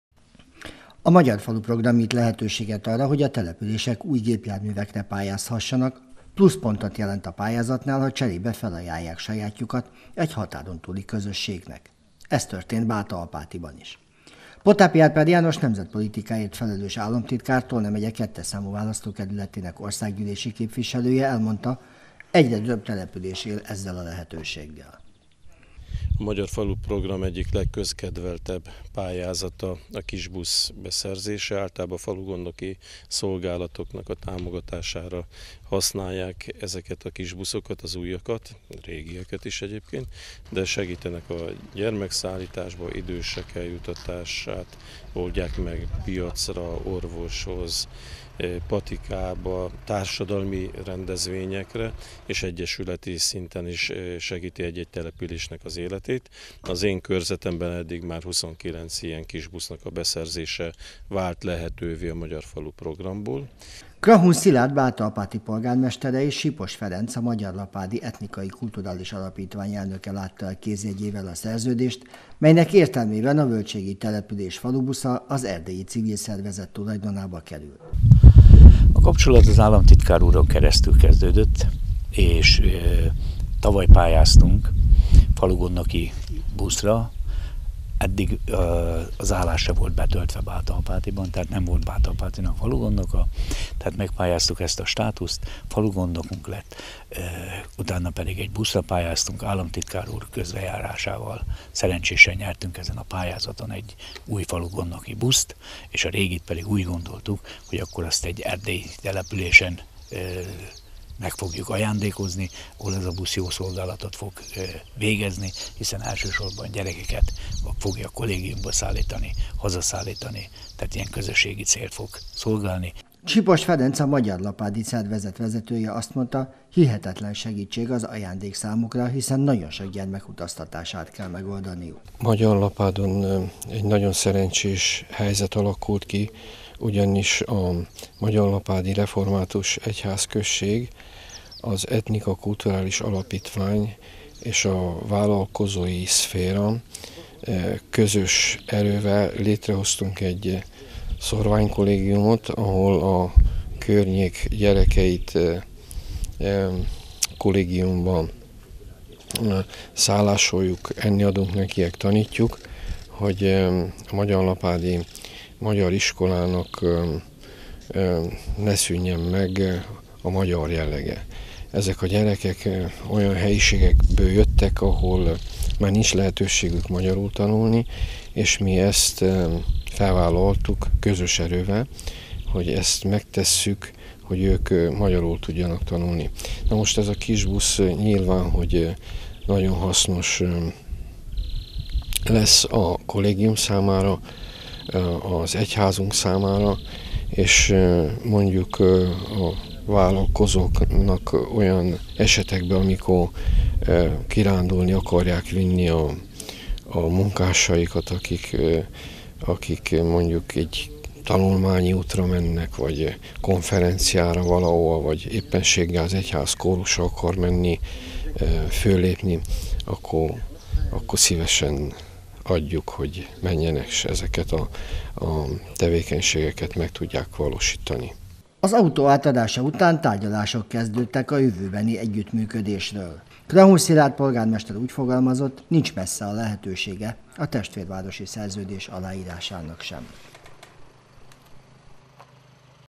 Buszcsere Bátaapátiban - a régi jármű Erdélyben teljesít majd szolgálatot
A Magyar Falu programnak köszönhetően ezúttal Bátaapáti nyert új kisbuszt, amivel a falugondnoki szolgálat látja el a jövőben a napi feladatokat. A település régi járműve a határon túlra kerül, az Erdélyi Magyarlapádon teljesít a továbbiakban szolgálatot. Potápi Árpád János nemzetpolitikáért felelős államtitkár, Tolna megye 2 sz. választókerületének országgyűlési képviselője azt mondta, a Magyar Falu program legsikeresebb része a falubusz pályázatoké.